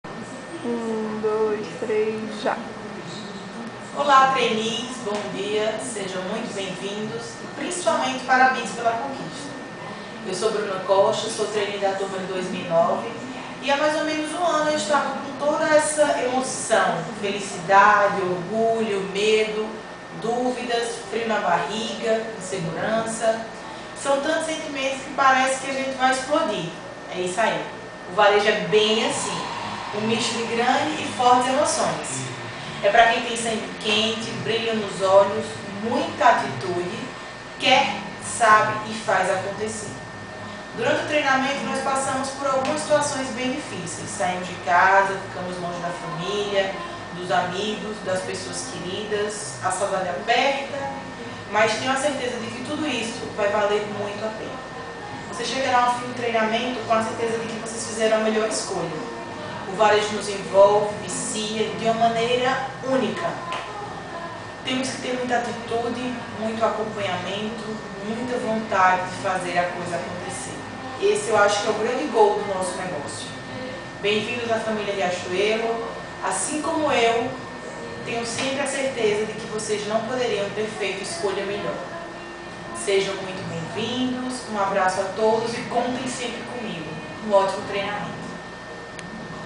Bruna -- Trainee Riachuelo 2009
Bruna Costa, trainee da turma de 2009 na Riachuelo, manda sua mensagem aos novos candidatos ao programa.